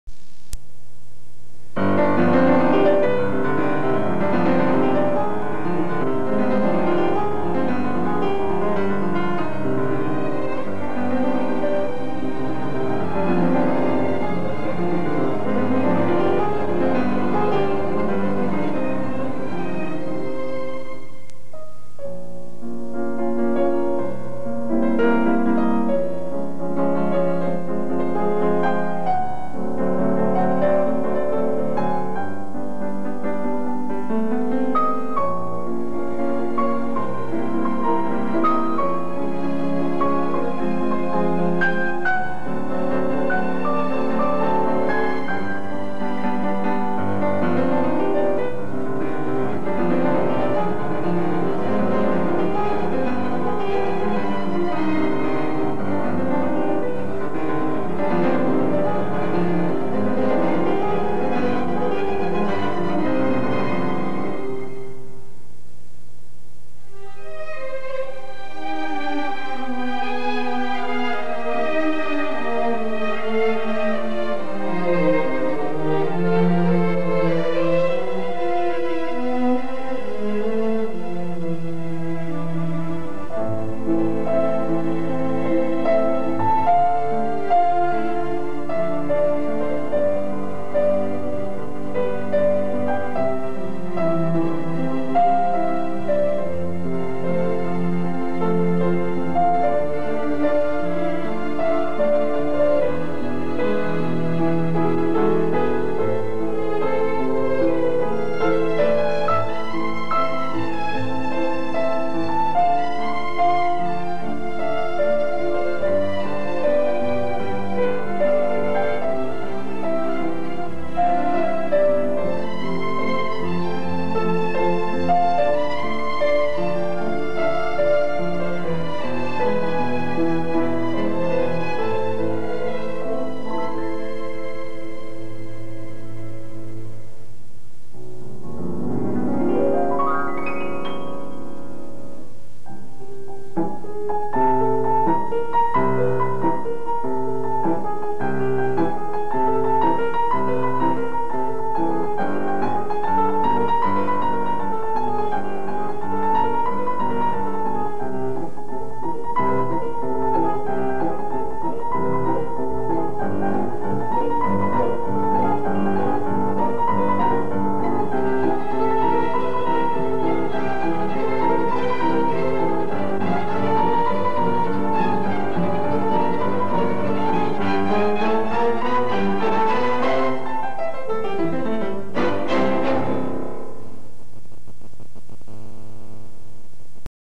Eduard's composition for Mazda Alpine Cycle
Eduard Volchek performs his composition with orchestra written especially for Mazda Alpine Cycle - April 1992